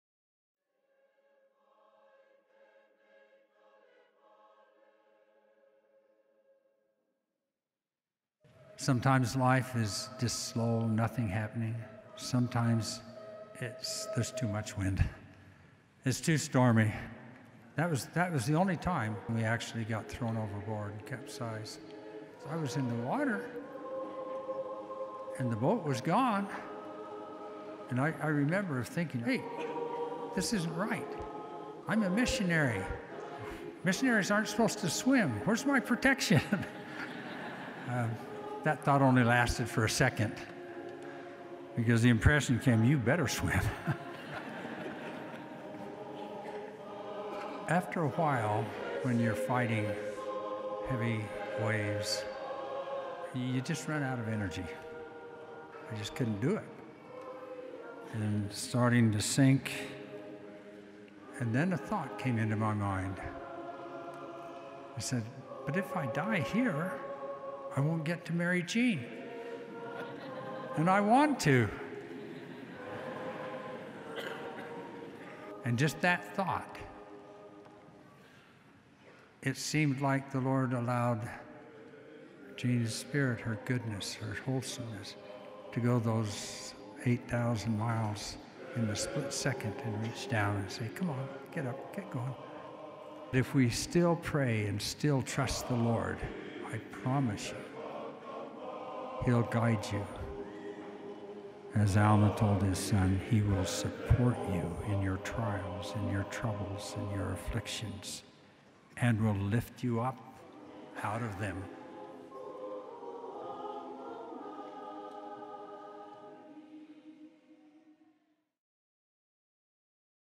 A Tongan Tabernacle Christmas with Elder John H. Groberg - "The Storm"
Thank you LDS Living & LightTheWorld for sharing this wonderful Christmas message featuring John H. Groberg and a Tabernacle full of faithful, musical Tongan Saints!

In this special message Elder Groberg tells us the story of the fateful day he was thrown overboard in a tropical storm and nearly drowned. You may remember this story because it’s a scene from OtherSideOfHeaven! No matter how perilous and overwhelming our trials, if we pray and -- and act -- in faith, the Lord will lift us out of them!

GIFT IDEAS To give the gift of Elder Groberg’s books as well as the 15th Anniversary Collector’s Edition DVD